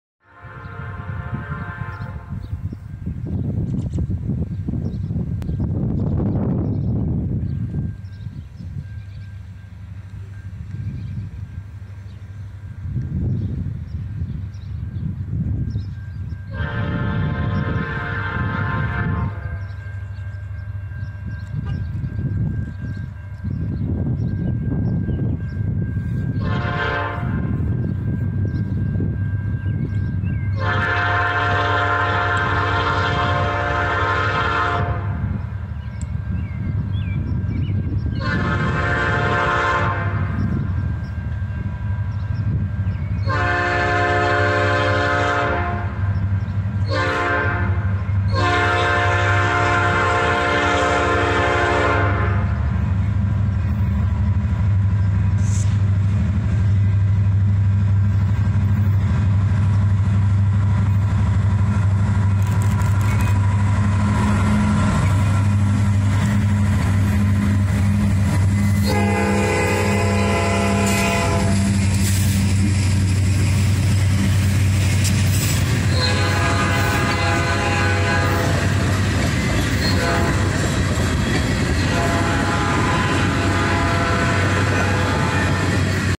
IC 1026 North - Peotone, IL
IC SD70 1026 leads a very colorful CN A407 North through Peotone.